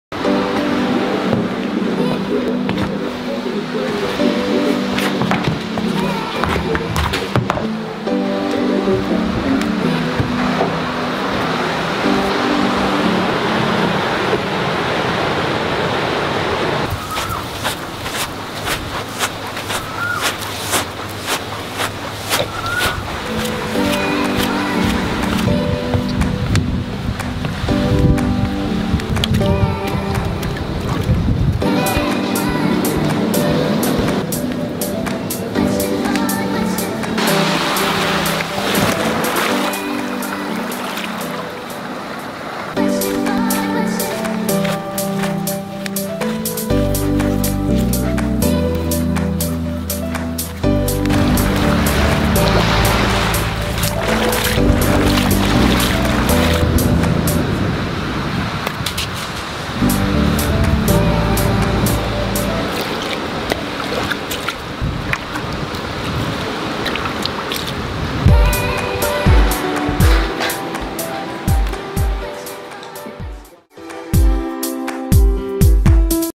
Living on an Island. The Maldives.
During our vacation at the Kuredu island resort and spa, I walked around bare foot for the whole stay. And I loved it!
That's why I got the idea to make a short movie about it.

PS: this is not a Vlog.